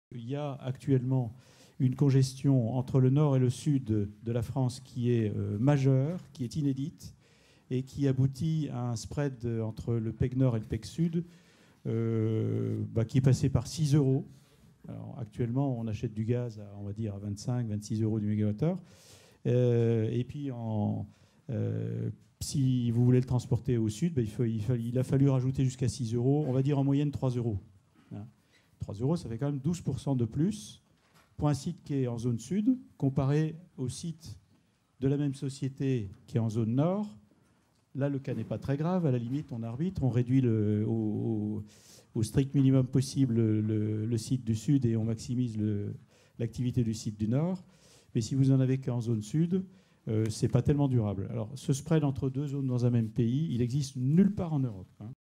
Le casse-tête du spread Nord-Sud dans le gaz - Claude Conrard
L'écart de prix entre les PEG Nord et Sud, de trois euros le MWh en moyenne au premier semestre 2012, a fortement perturbé les conditions d'achat de gaz des entreprises situées dans la zone Sud. Suscitant le mécontentement des gazo-intensifs, explicite le président de la commission pétrole et gaz de l'Uniden, Claude Conrard, également directeur des Affaires publiques Energie France de Solvay.